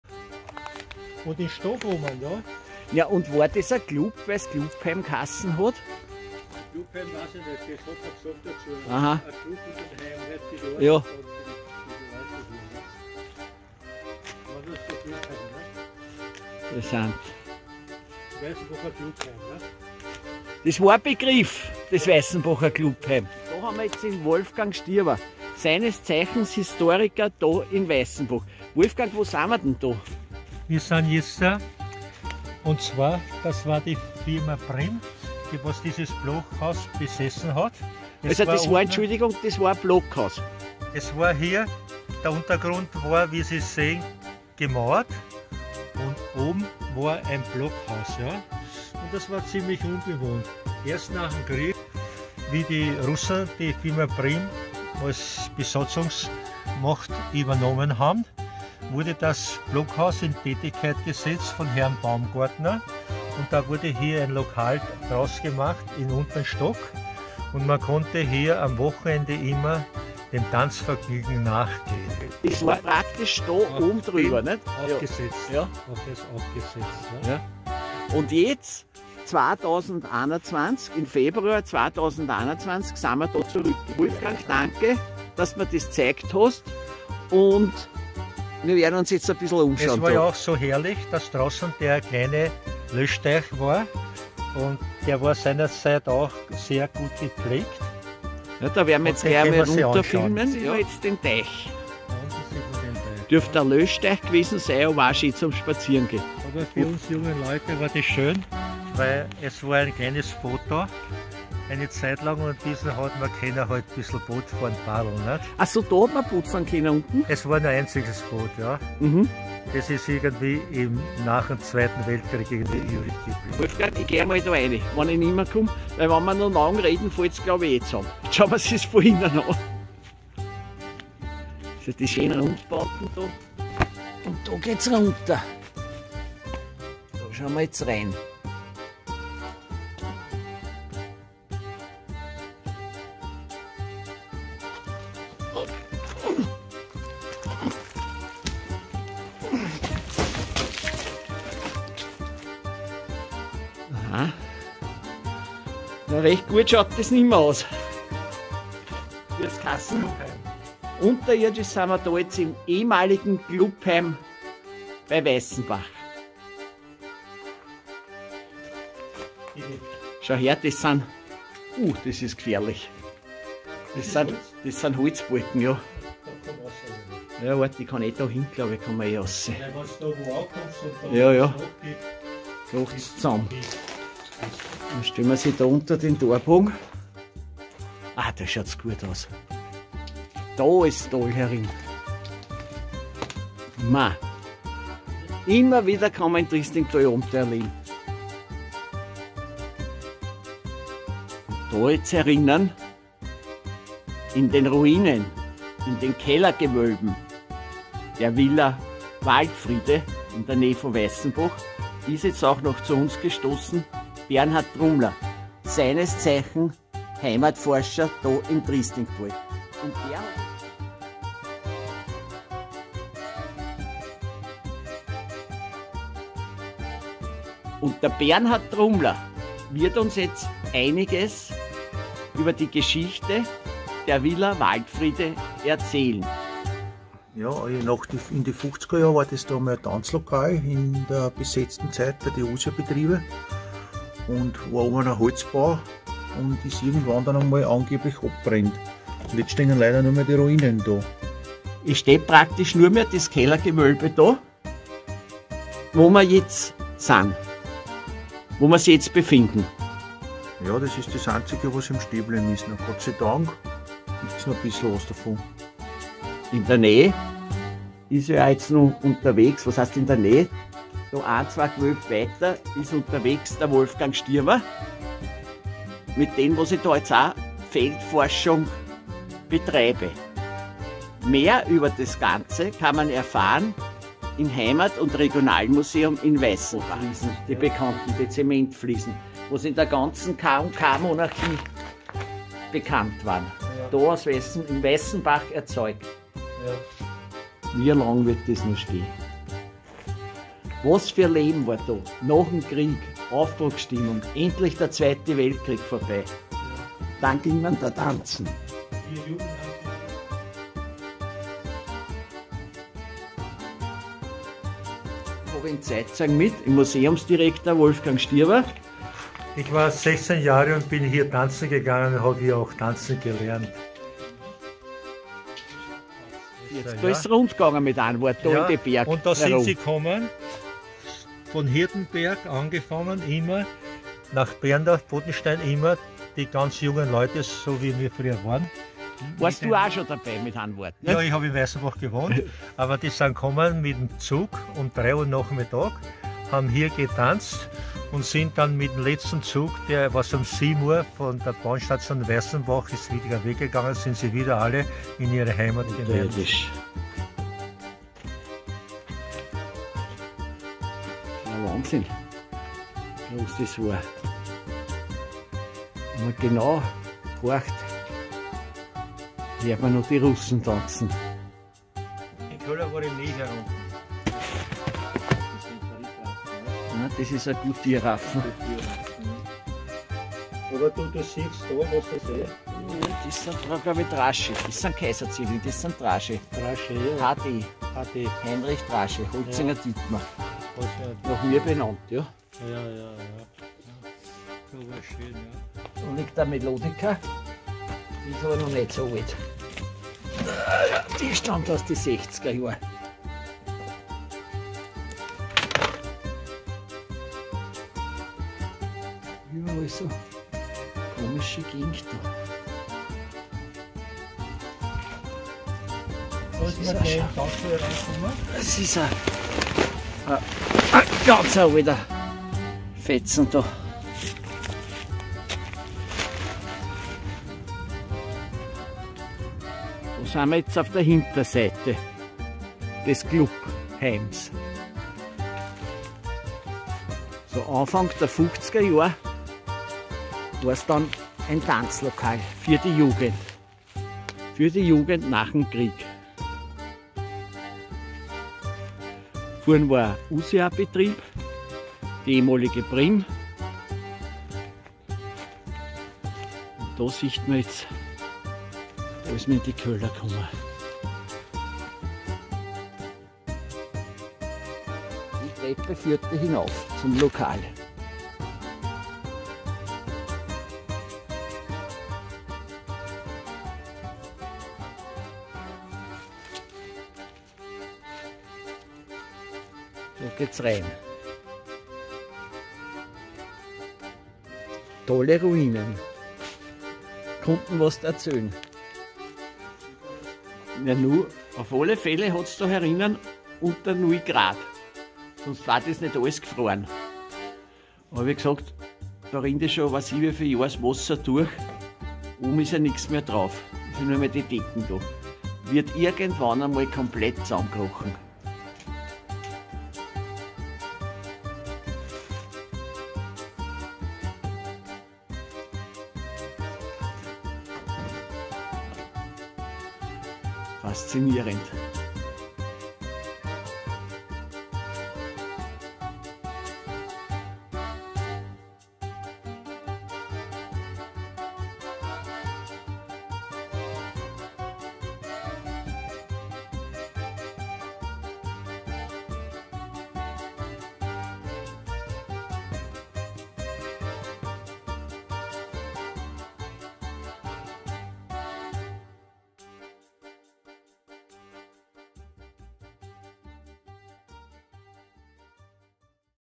NUR DAS UNTERIRDISCHE GEWÖLBE DER VILLA WALDFRIEDE-USIA-KLUBHEIM PRYM, BLIEB ERHALTEN - 50IGER JAHRE
LOST PLACES + VILLA WALDFRIEDE + USIA CLUBHEIM PRYM: Mit Museumsleiter Wolfgang Stiawa und Heimatforscher Bernhard Trumler begab sich TRIDOK-Chef Dietmar Holzinger auf Feldforschung. Untersucht wurden diesmal die unterirdischen Reste des USIA-Klubheimes, vormals Villa Waldfriede bei Weissenbach/Triestingtal.
EIN DANKESCHÖN FÜR DIE BEREITSTELLUNG DER DES HISTORISCHEN FOTOMATERIAL AN HEIMATMUSEUM WEISSENBACH UND ARCHIV BERNHARD TRUMLER.